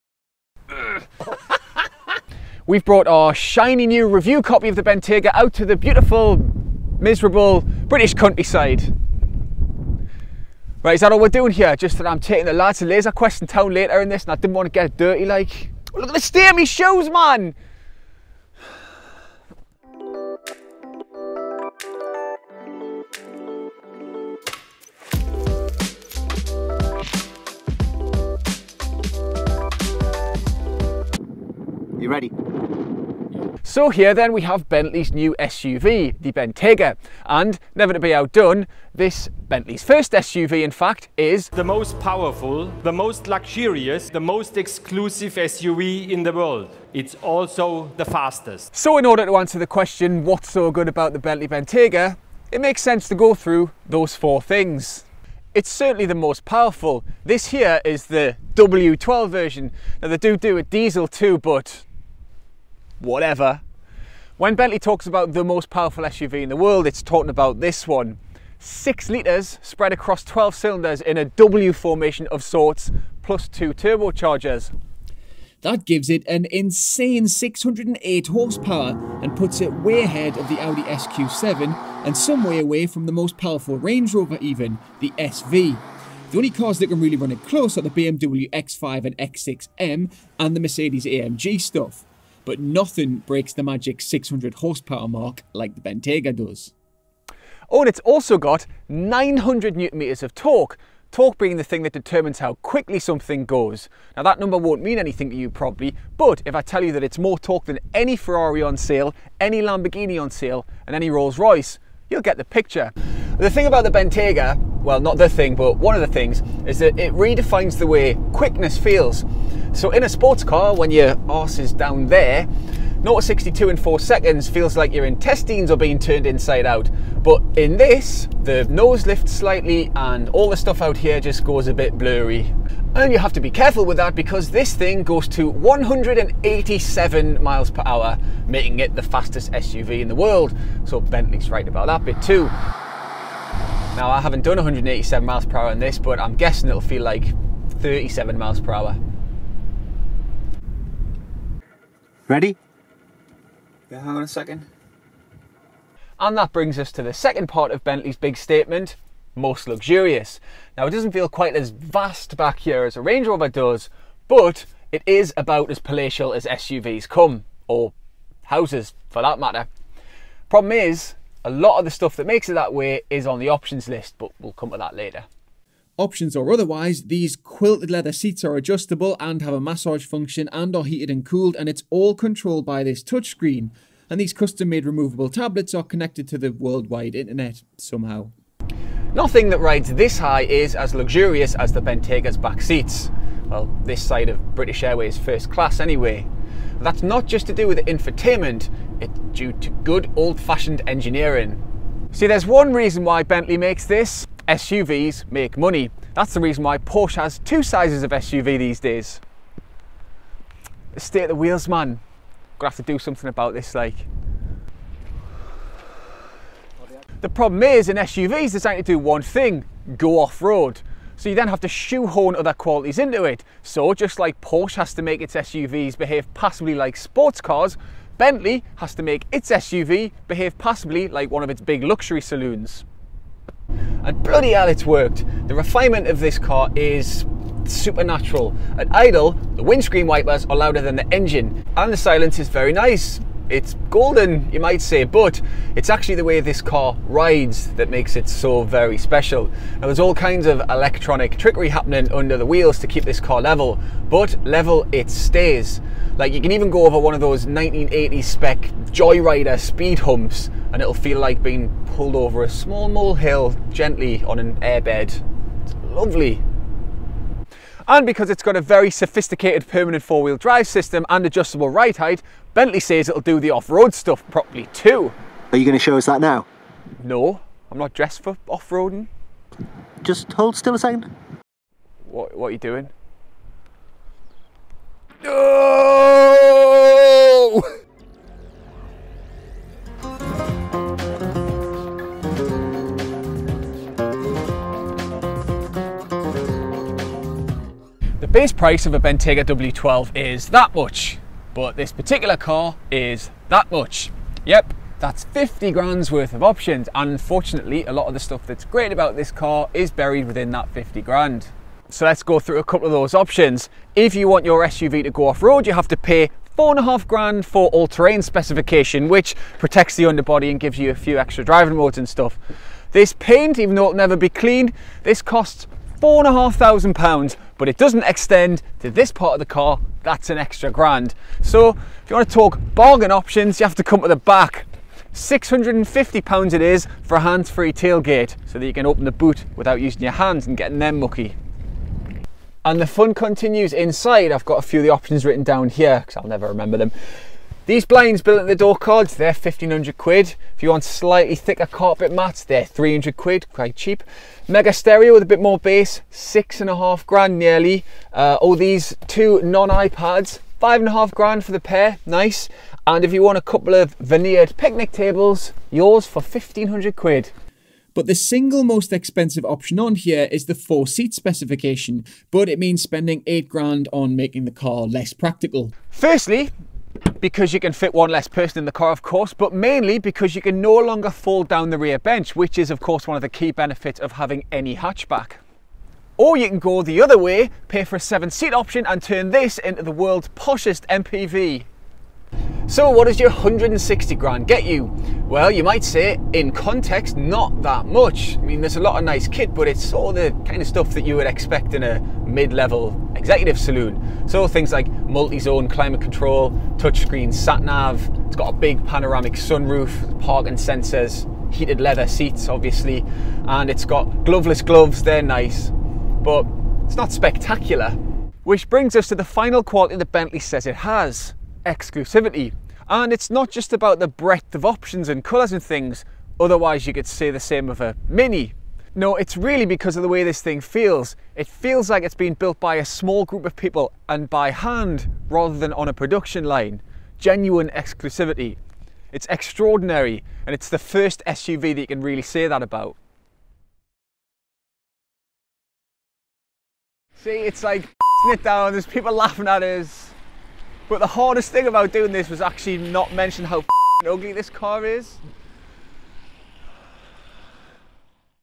Car review | What's so good about the...2018 Bentley Bentayga?
When Bentley does an SUV, Bently makes golly darned sure it's the most spiffing blasted SUV there is, chap.

Here's what all the bloody fuss is all about. Tally ho.